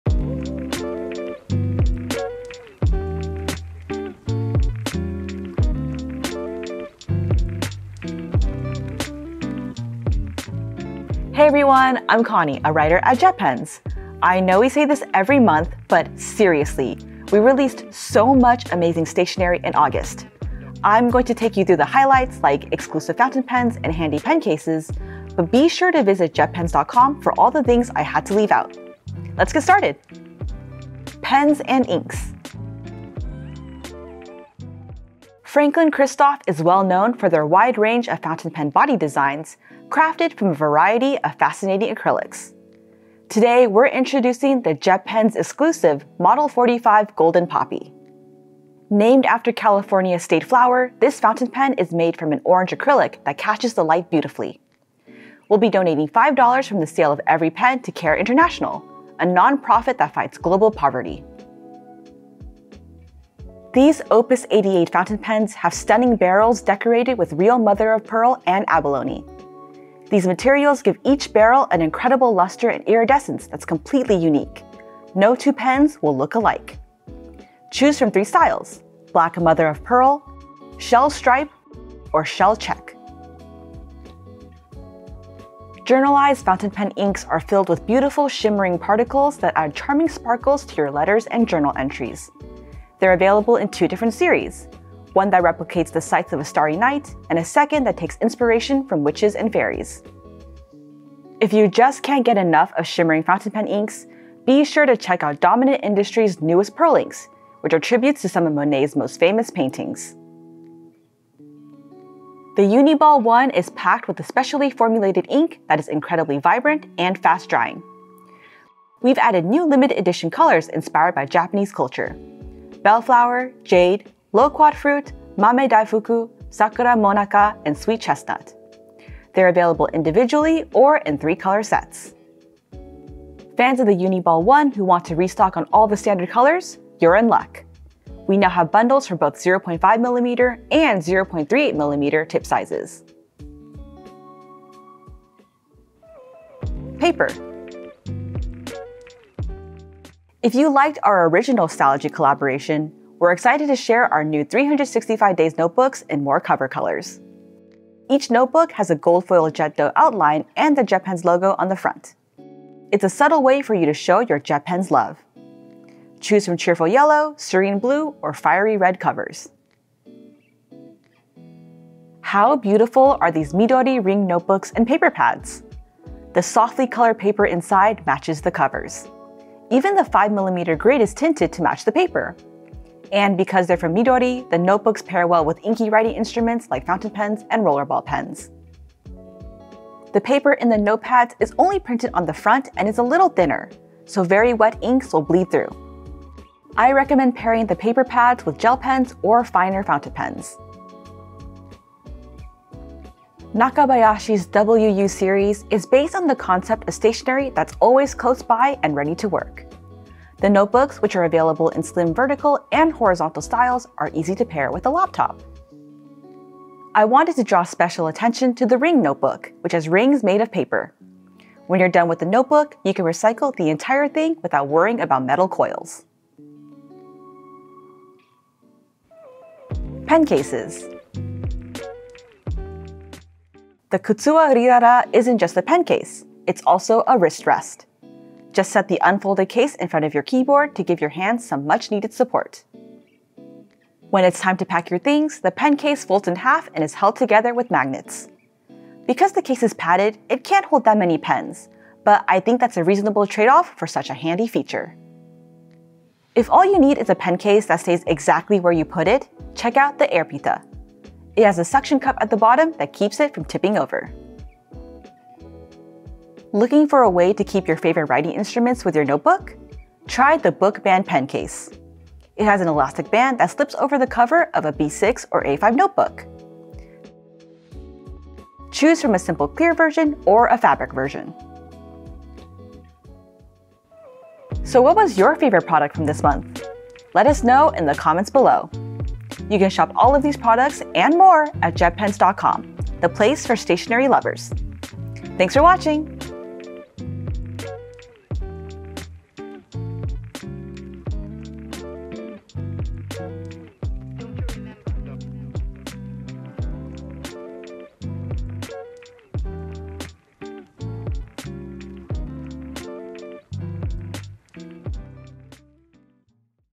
What’s NEW at JetPens? | August 2023 STATIONERY Round-up! New Stalogy x JetPens Notebook Colors! ✨📓
or click More to see individual product links. Check out our favorite stationery releases from August 2023, including exclusive fountain pens and notebooks, limited edition gel pens, adorable pen cases, and more.

▬▬ ✦ P R O D U C T S  &  T I M E S T A M P S ✦ ▬▬

0:00 Intro
0:30 Pens & Inks
2:26 Paper
3:49 Pen Cases
4:44 Outro